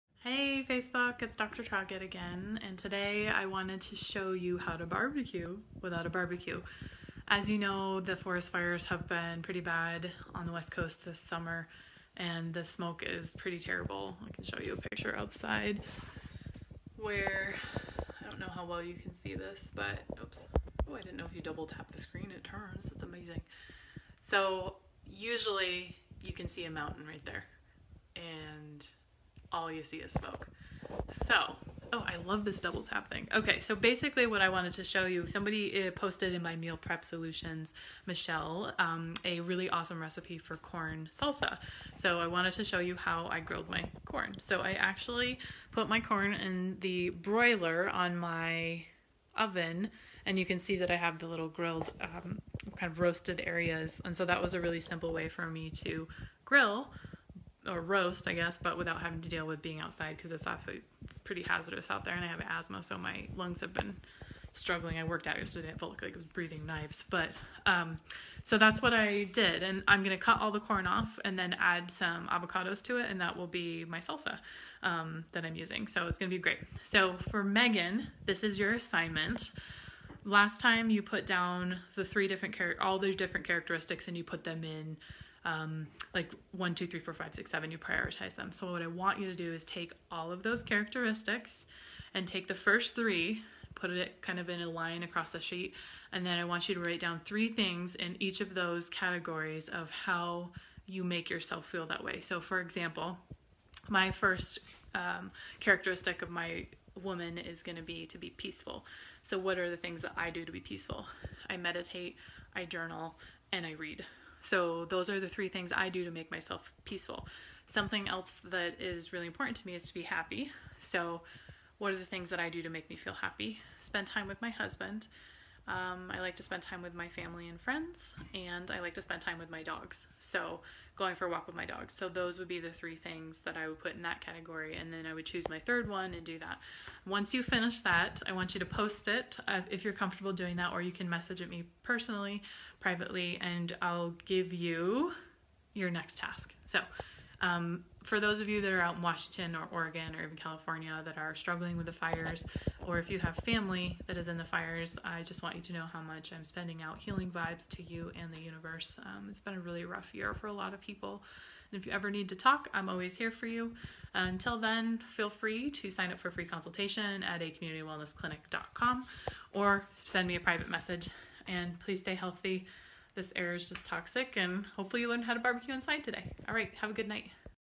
How to BBQ Inside (Without a BBQ)
There is nothing worse than wanting to BBQ - but what if it's raining? What if you don't have a BBQ? Don't worry, I got you!